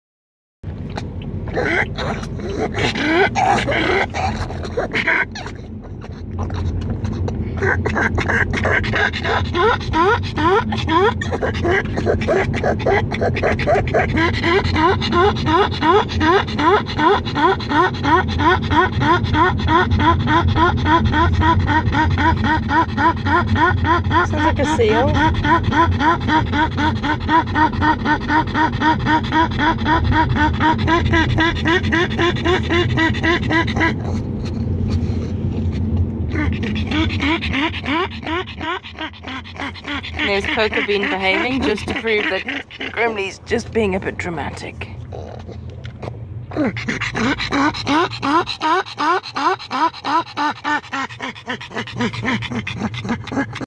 Pug over excited in the car
Grimley gets so excited in the car and ends up sounding like a seal... the air con is on and the car is cool so he's not hot just very excited.